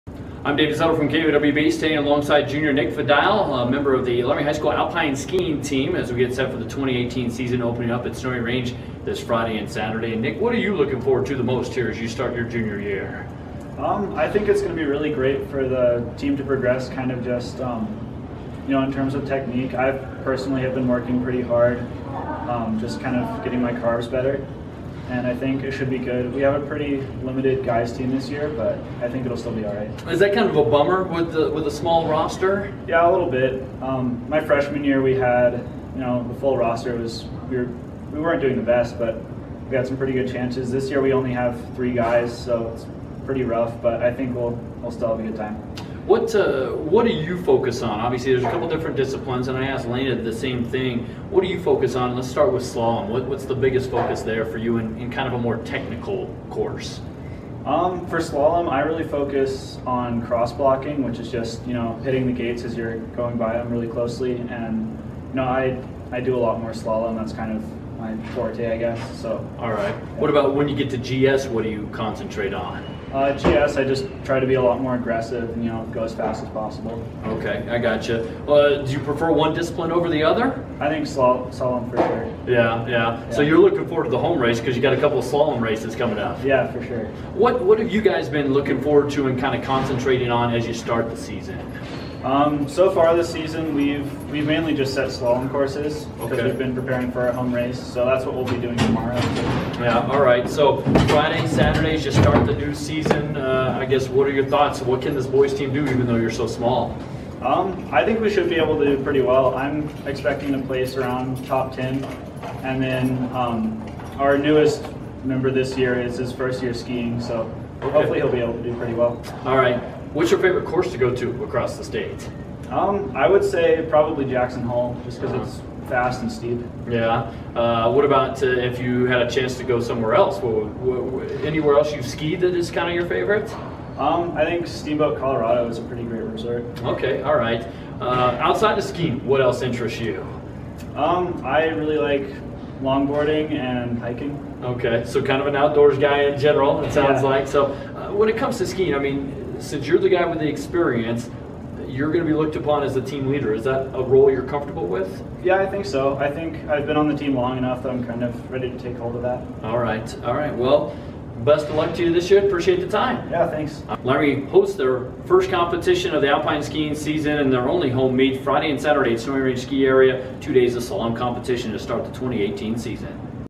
Laramie Alpine Skiing Preview With Junior Nick Fadial
The 2018 high school alpine skiing season begins in Laramie Friday and Saturday.

Junior Nick Fadial is the leading returning performer on the slopes for the Plainsmen from last year.

Fadial talked with KOWB's David Settle about the upcoming season, being the veteran on a small boy's team, what he's doing in training and more.